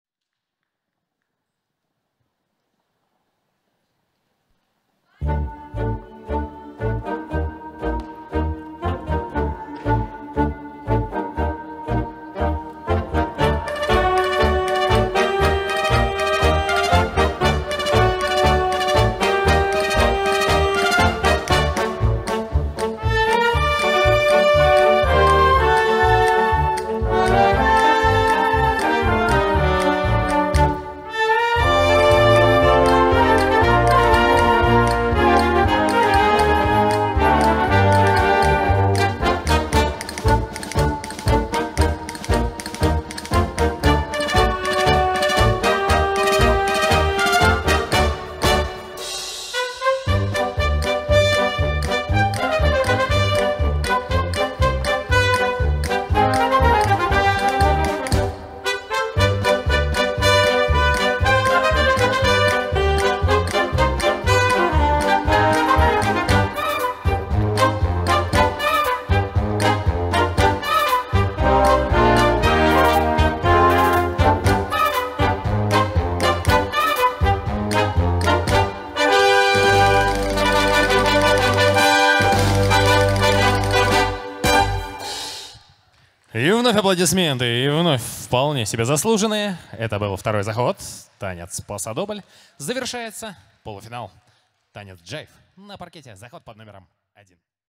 ☂Молодежь 1 Rising Stars (до 19 лет)(Open) PasoDoble(PD) ПолуФинал LA(5) Capital Cup Minsk – 2021
Capital Cup Minsk – 2021 Международные соревнования по спортивным бальным танцам
дата проведения: 16-17 октября 2021 года
Организаторы:

Couple №128: Тхорев Владислав и Буглай Кристина
(Club: Арабеск, г. Витебск, Беларусь)
Trainer (s): Хамутовская Ольга Леонидовна